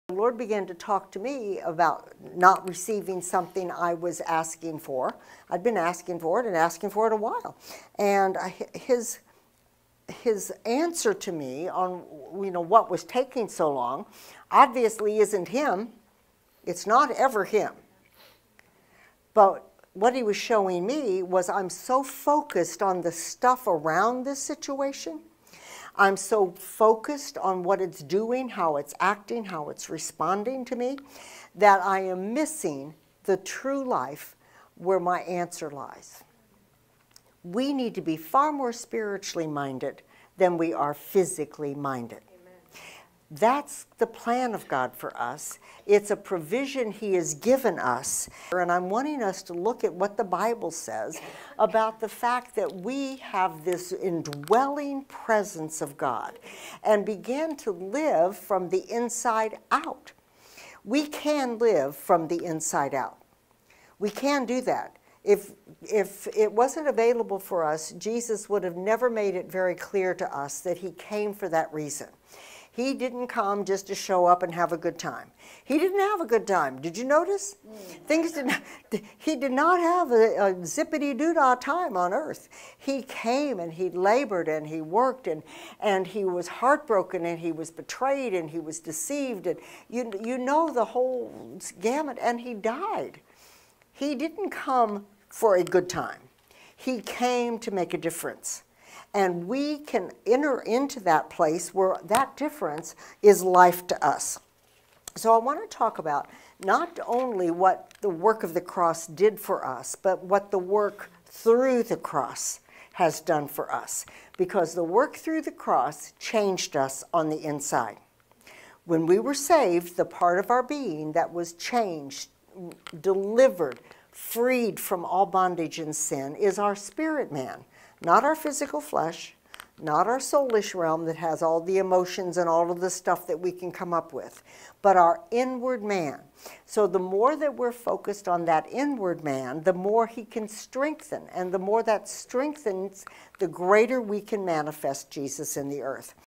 The STUFF Around the SITUATION
The Lord began to talk to me about not receiving something I was asking for. I'd been asking for it and asking for it a while, and His answer to me, what was taking so long, obviously isn't him, it's not ever Him. What He was showing me was I'm so focused on the stuff around this situation, on what it's doing, how it's acting, and how it's responding to me, that I am missing the true life where my answer lies... [WATCH NOW]